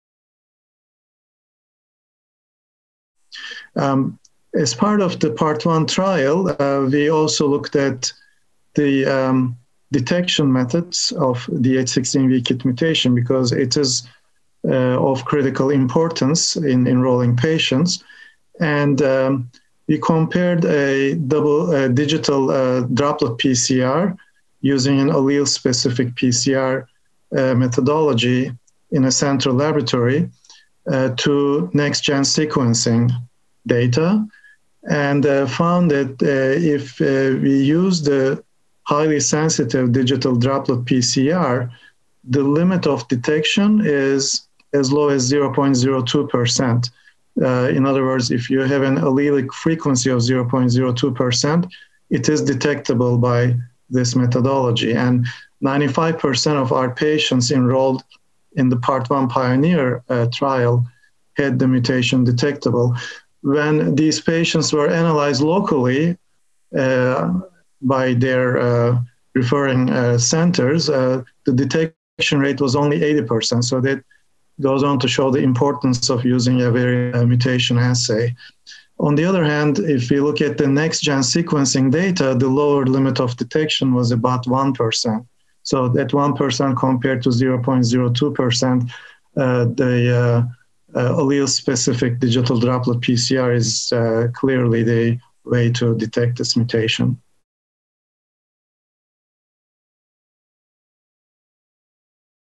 ddPCR detects KIT D816V mutations with high sensitivity in systemic mastocytosis
Cem Akin, MD, PhD, University of Michigan, Ann Arbor, MI, discusses the results of a study which investigated the limit of detection of the KIT D816V mutation by digital droplet polymerase chain reaction (ddPCR) or by next-generation sequencing (NGS) of the peripheral blood samples, in patients with indolent systemic mastocytosis. Detection of the mutation, present in around 65% of all systemic mastocytosis cases, is important to confirm the clinical diagnosis and the allelic fraction can be of prognostic use. Samples from participants of the PIONEER trial were analyzed and the results showed ddPCR to be highly sensitive, with a limit of detection of 0.02%: a 30-fold greater sensitivity over NGS which had a lower limit of detection of 1%. The results suggest a minimally invasive ddPCR assay could be used as a screening tool for mastocytosis, including in patients with a low abnormal mast cell burden. This interview took place during the 62nd American Society of Hematology (ASH) Annual Meeting and Exposition, 2020.